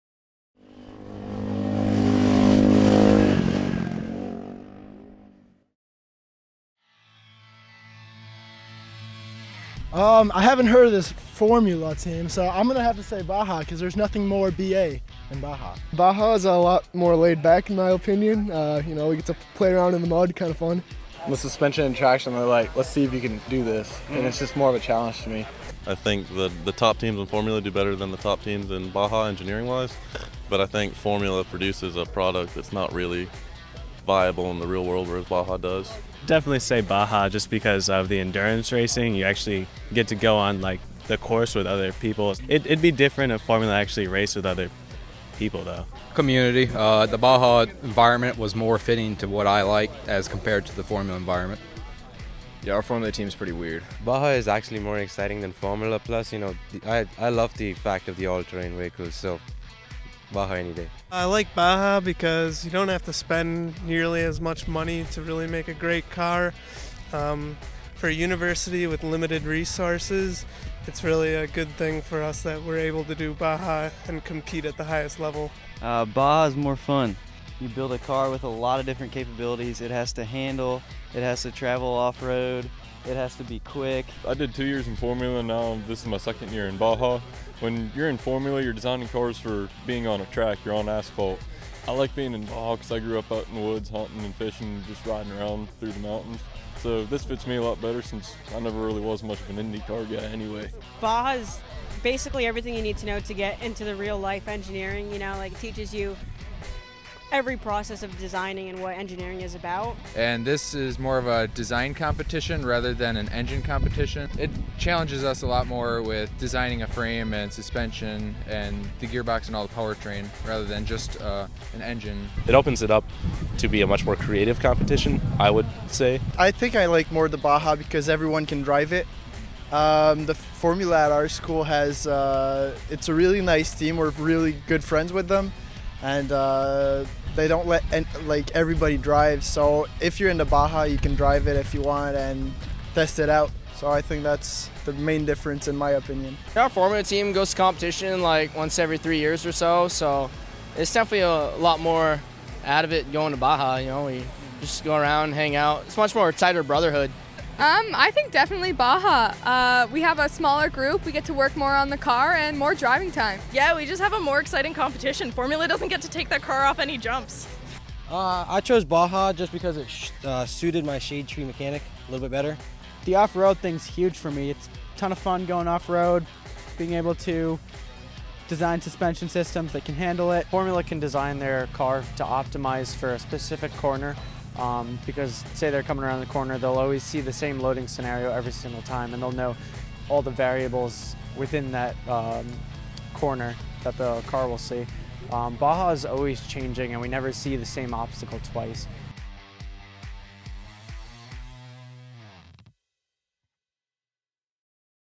Baja vs Formula
We spoke to Baja SAE competitors about the difference between Baja SAE and Formula SAE.  While some answers were lighthearted, others highlight some of the key differences in the engineering design challenge of both competitions.